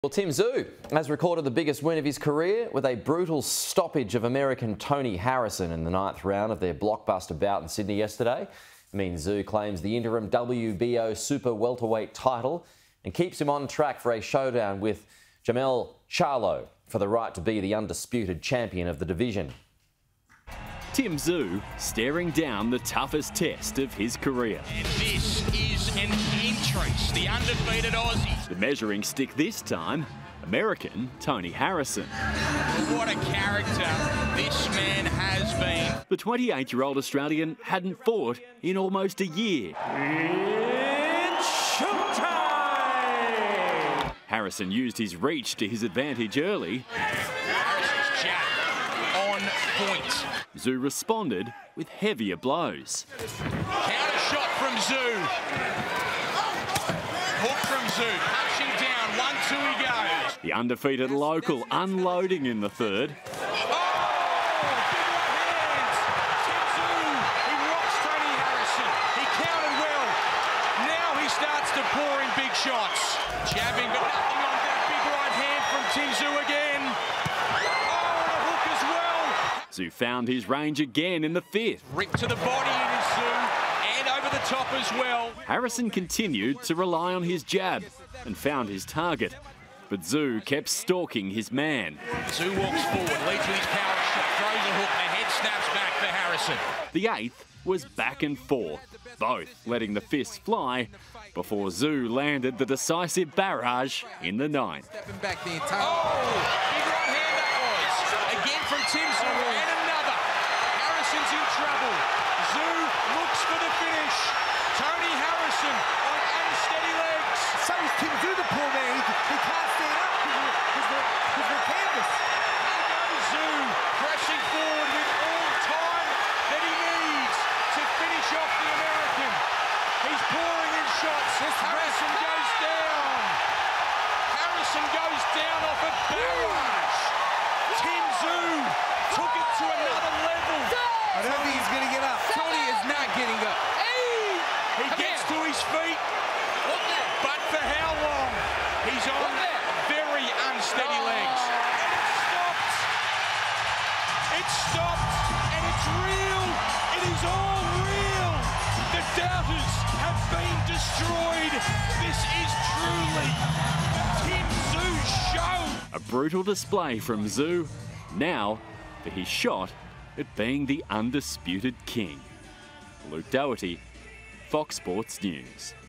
Australian boxer Tim Tszyu wins world title
Australian boxer Tim Tszyu has recorded the biggest win of his career with a brutal stoppage of American Tony Harrison in the ninth round of their bout in Sydney.